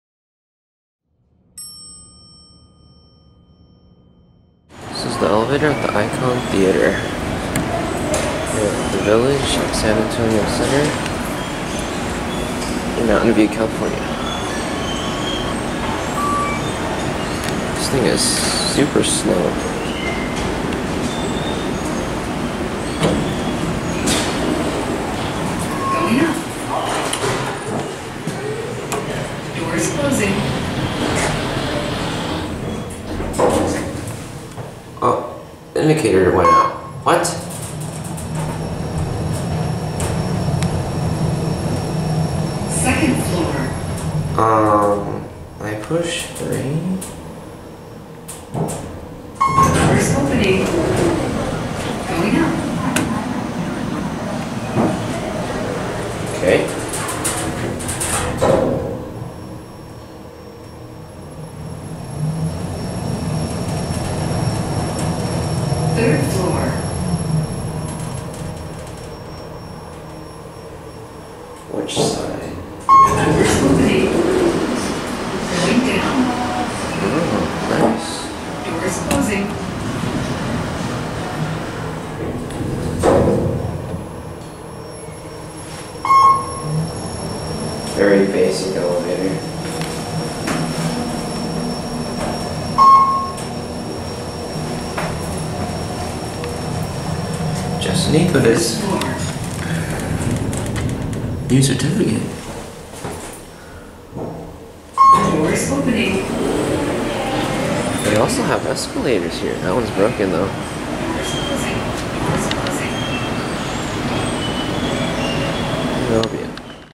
KONE EcoDisc Traction Elevator - ShowPlace ICON Theatre & Kitchen - Mountain View, CA
[Recorded 12-20-22] This is just a basic KONE. I do like how it announces when the doors are opening and closing though. You don't really hear those announcements a whole lot. This is a really big theatre and they have escalators too. This elevator was labeled as Car 2 so naturally I went looking for Car 1. I couldn't find it so I'm assuming it was a service elevator or something.